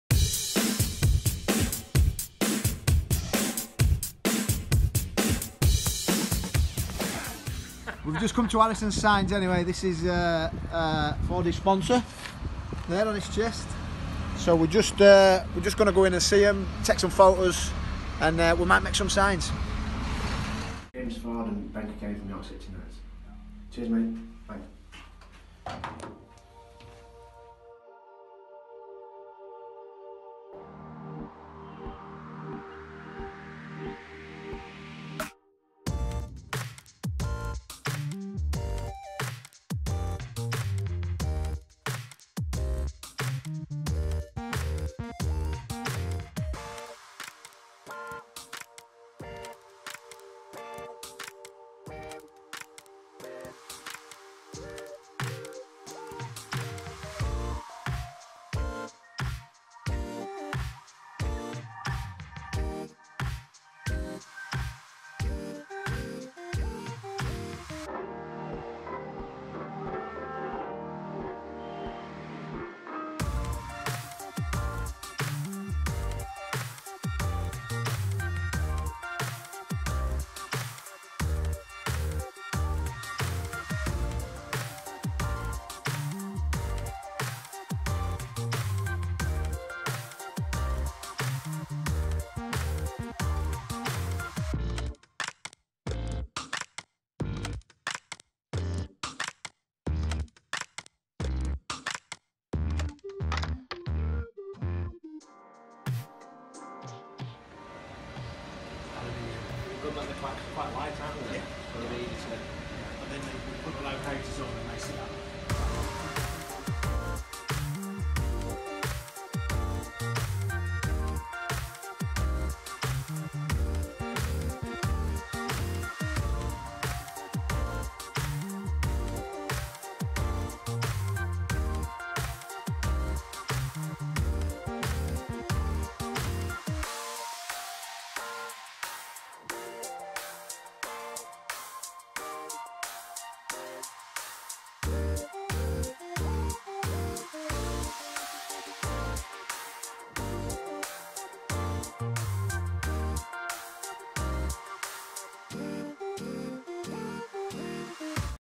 York City Knights and Harrisons Signs
While there, the boys saw Harrisons developing a new sign with new branding for the club's principal partner, York St John University.